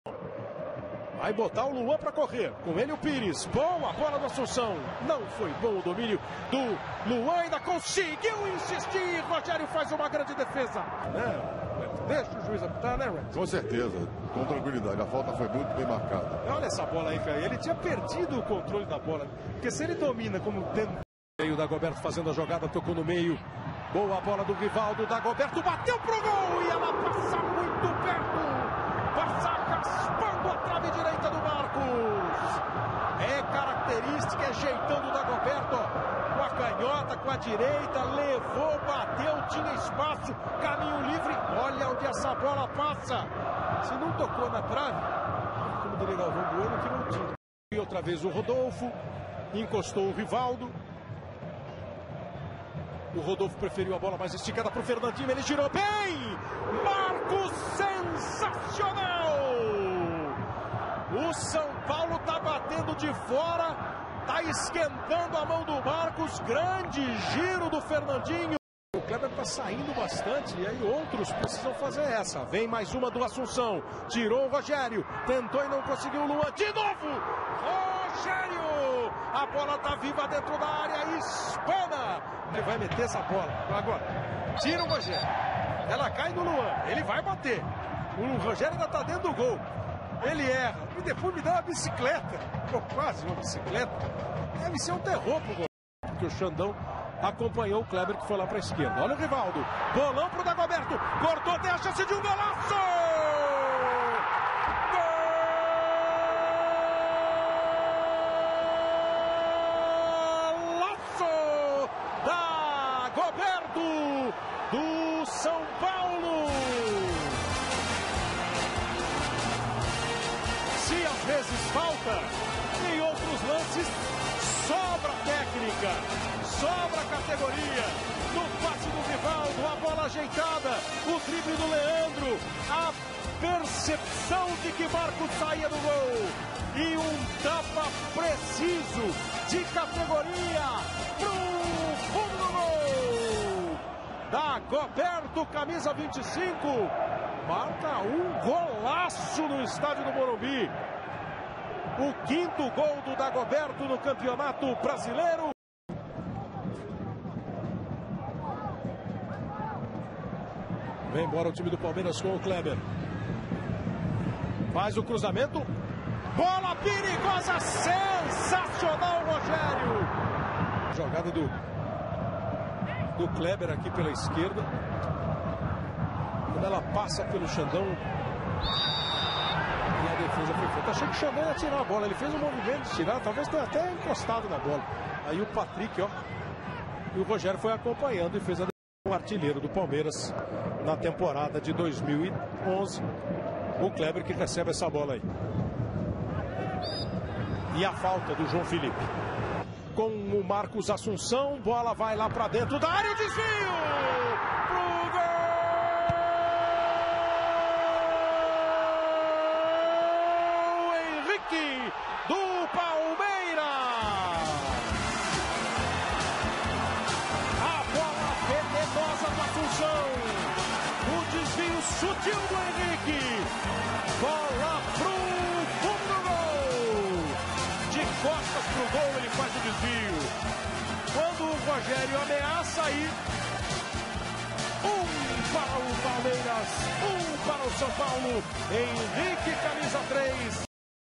São Paulo 1 x 1 Palmeiras - Brasileirão 2011
18a rodada - 21/08/2011

Domingo de muito frio na capital paulista. Dois rivais tradicionais que entraram em campo mais preocupados em não perder do que em ganhar. Apesar das várias peças de qualidade, São Paulo e Palmeiras maltrataram a bola e não mereceram mais do que o empate por 1 a 1, resultado que deixou ambos em situações inalteradas na tabela. O Tricolor, que não aproveitou o tropeço do líder Corinthians no sábado, segue na terceira colocação, com 34 pontos. Já o Verdão, que manteve seu fraco desempenho como visitante, é o sexto, com cinco pontos a menos. O público também não foi animador: 16.813 pessoas pagaram para ver o duelo, com renda de R$ 587.600.